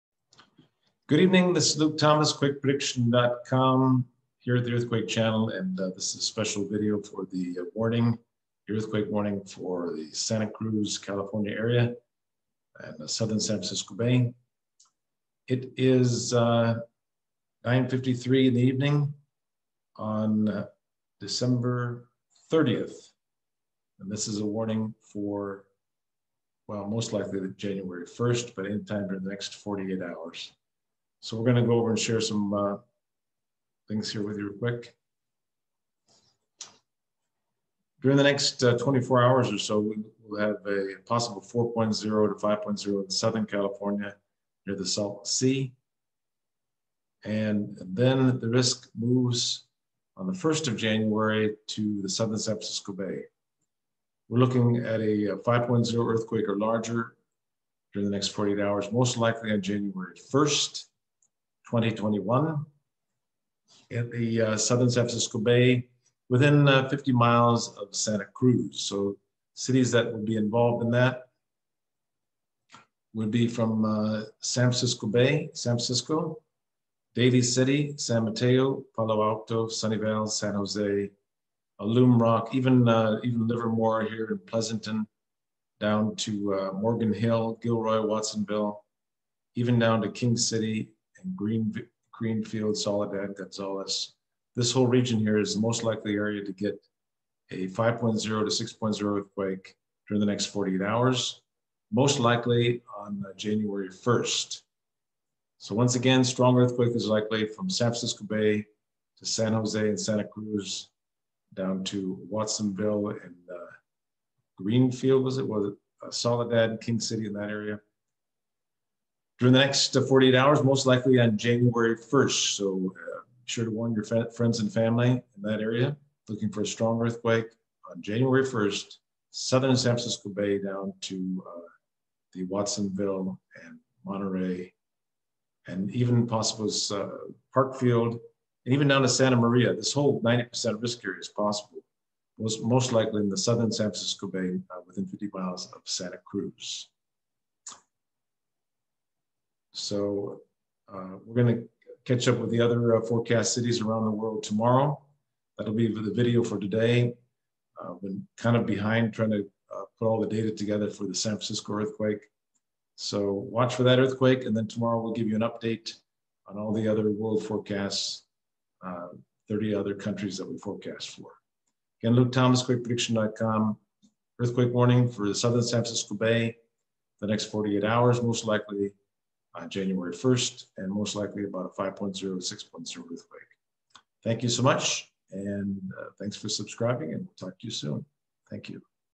San Francisco Earthquake Warning: 5.0 to 6.0 earthquake is likely within 50 miles of Santa Cruz, CA
San Francisco Earthquake Warning: 5.0 to 6.0 earthquake is likely within 50 miles of Santa Cruz, CA - 01/01 to 01/02